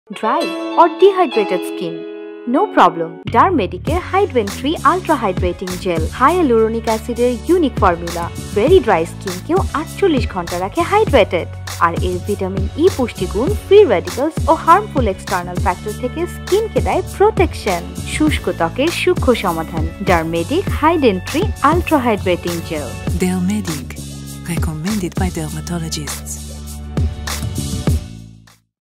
Dermedic Ultra Hydrating Cream Gel এ শুষ্ক ত্বককে ৪৮ ঘণ্টা রাখুন হাইড্রেটেড !
আপনার স্কিন কি ড্রাই নাকি ডিহাইড্রেটেড? ব্যবহার করুন Dermedic এর Ultra Hydrating Cream Gel. যা আপনার স্কিনকে ৪৮ ঘণ্টা রাখে ময়েশ্চারাইজ। ফলে স্কিন হয় স্নিগ্ধ ও লাবণ্যময়।

হায়ালুরোনিক এসিডের গুণে এই ক্রিম জেল স্কিনকে করে মসৃণ। আর ভেতর থেকে স্কিনকে রাখে সতেজ ও স্বাস্থ্যকর। এতে থাকা ভিটামিন ও মিনারেল, স্কিনের প্রয়োজনীয় পুষ্টি ও জলের চাহিদা পূরণ করে, আর স্কিনকে দেয় দূষণ থেকে সুরক্ষা!